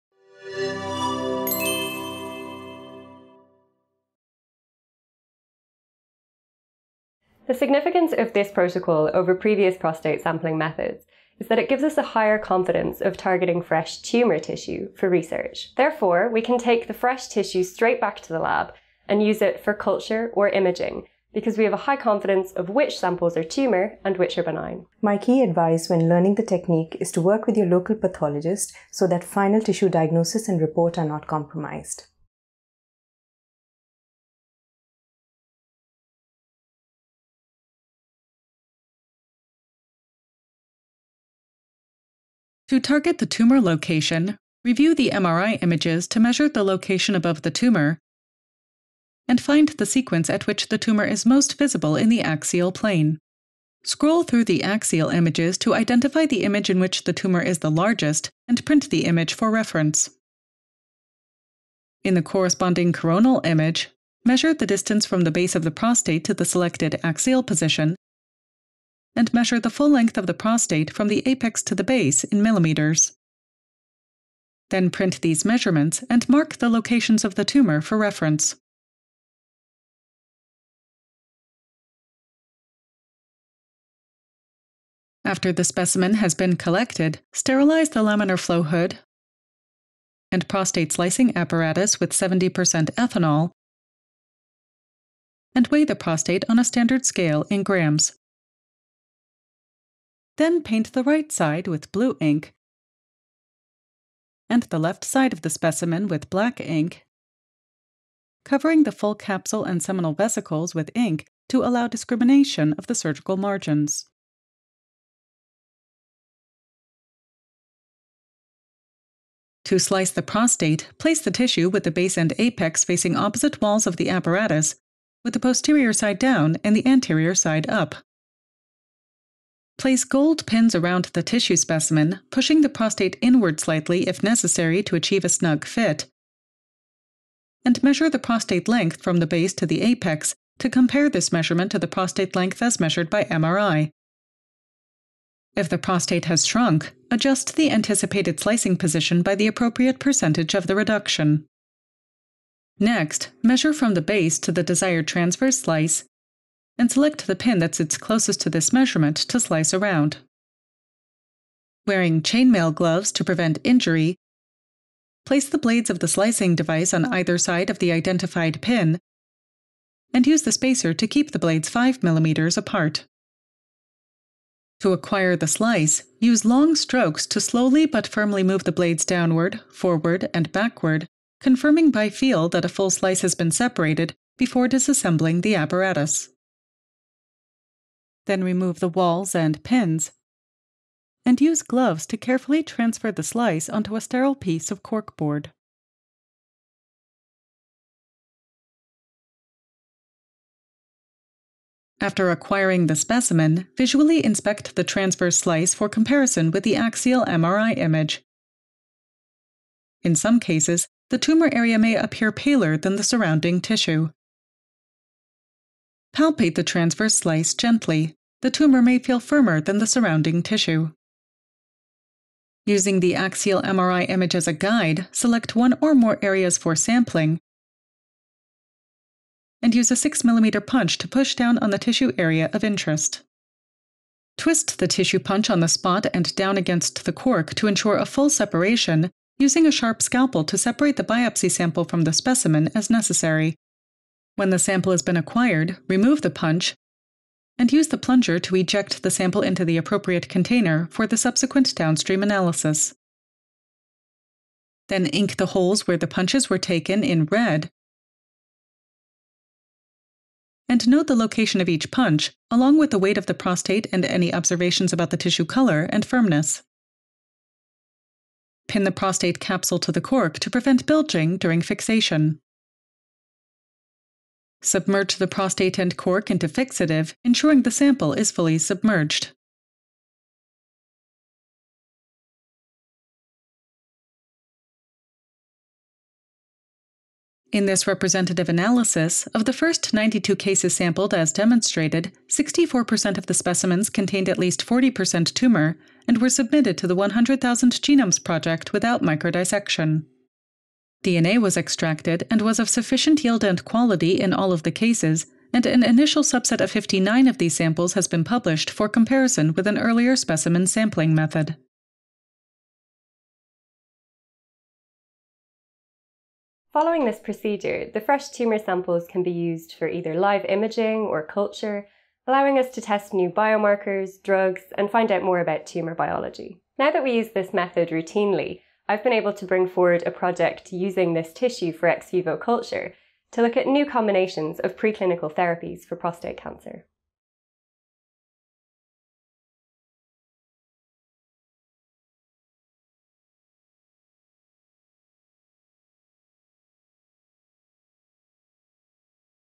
PEOPLE: Using MRI to target areas of prostate tumour tissue for biobanking.
Science methodology from our lab! This is our latest video, which we made for a publication with JoVE, The Journal of Visualized Experiments. It shows how we use MRI to target prostate tumours for biobanking. Many thanks to all our collaborators!